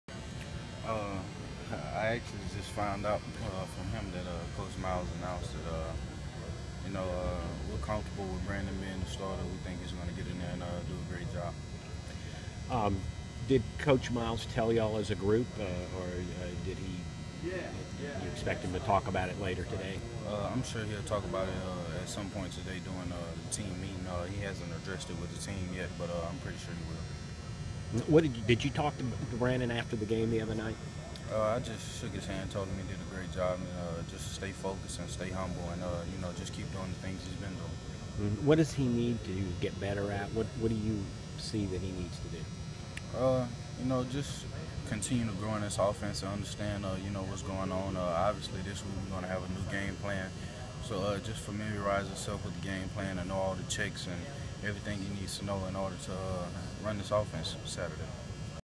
LSU RB Terrence Magee on Brandon Harris starting at QB
LSU running back Terrence Magee on freshman Brandon Harris starting at quarterback on Saturday against Auburn.